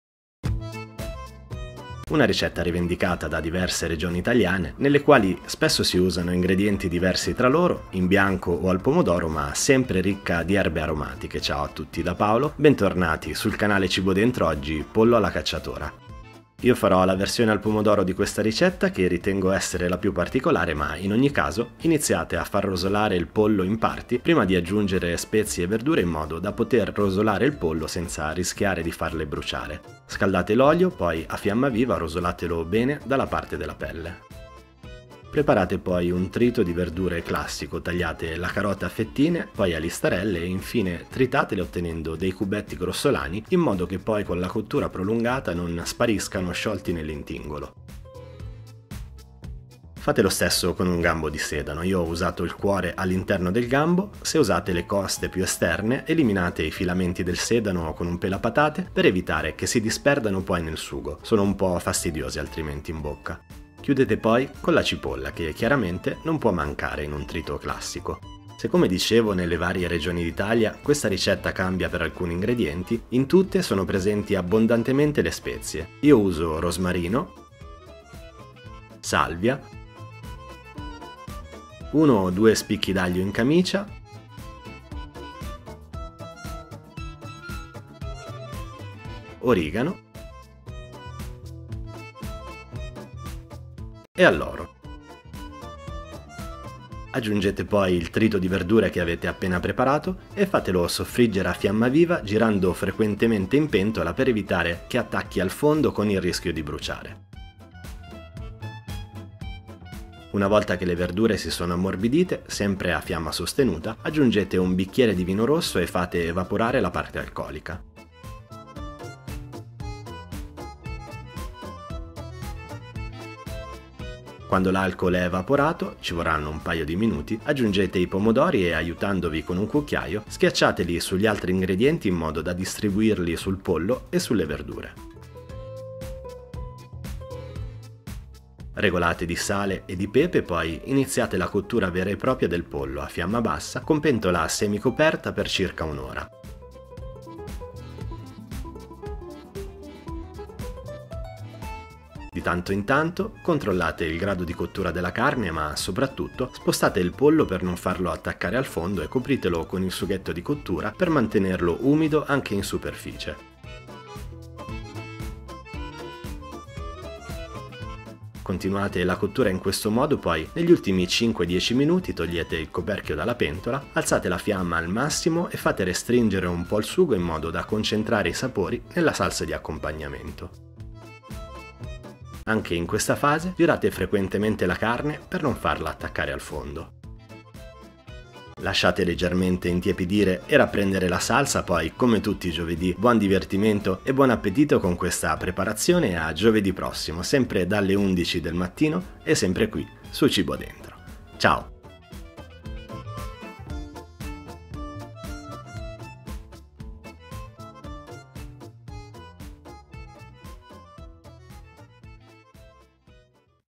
Pollo alla Cacciatora
NUOVI VIDEO TUTTI I GIOVEDÌ DALLE 11:00 DEL MATTINO
Pollo alla Cacciatora Classico - Facile e Saporito ricco di Erbe Aromatiche, un Classico della Cucina Italiana